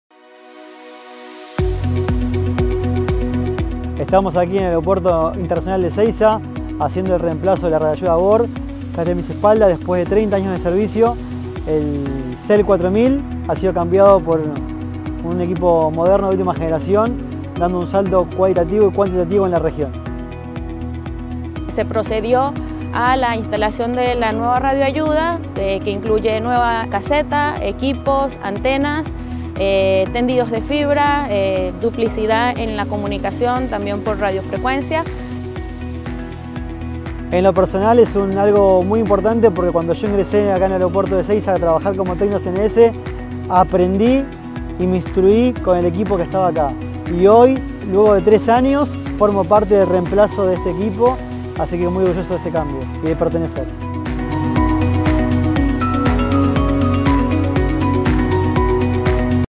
Nuevo VOR en el Aeropuerto de Ezeiza
El nuevo VOR del Aeropuerto de Ezeiza ya está instalado y operativo. ✈️

La incorporación del flamante radiofaro reemplaza al anterior con 32 años de servicio. Con mejor tecnología fortalece la Seguridad Operacional y permite una mayor disponibilidad.

El radiofaro VOR permite a las aeronaves volar de un punto a otro siguiendo una trayectoria trazada por el sistema.

Su modernización representa una notable mejora tecnológica, de equipamiento y conectividad para el Aeropuerto Internacional de Ezeiza.

Esta instalación llevada a cabo por EANA forma parte del PlanDeModernizaciónDelTransporte impulsado por el Ministerio de Transporte de la Nación 🇦🇷.